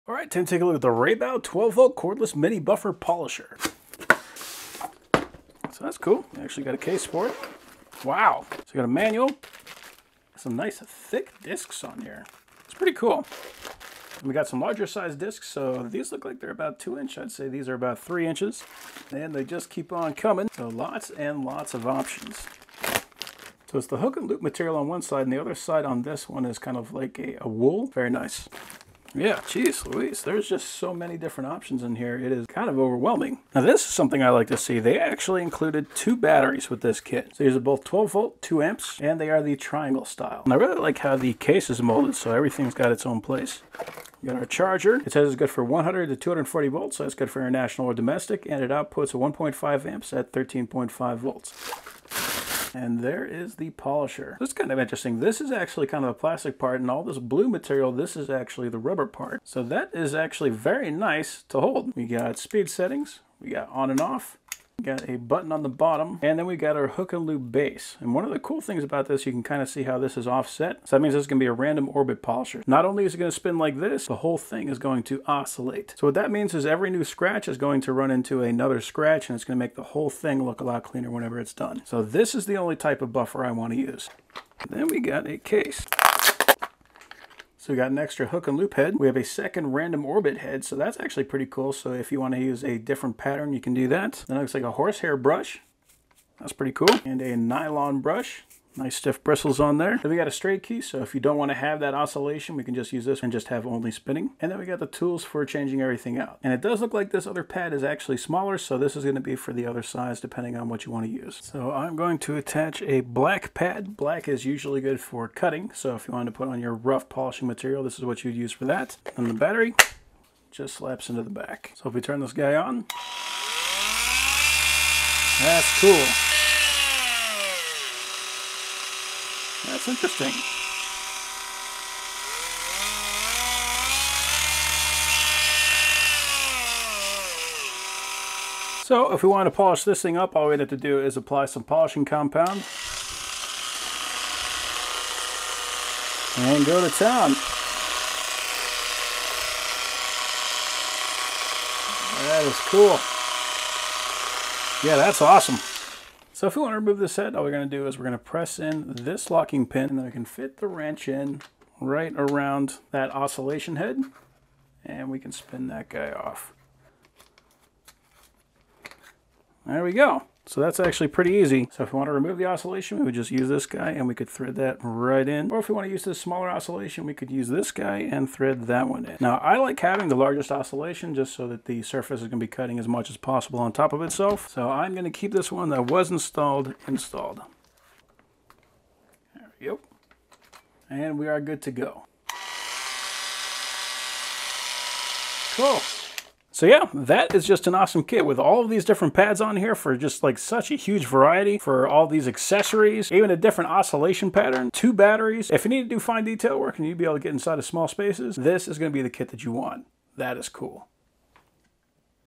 Mini Polisher, RAYBAO 12V Cordless Buffer Polisher
Mini Polisher, RAYBAO 12V Cordless
Buffer Polisher with 2 Batteries, 6 Speeds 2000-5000RPM, Works as DA & Rotary Polisher, Super Lightweight (2.16lb), Polisher for Car Detailing, Polishing, Cleaning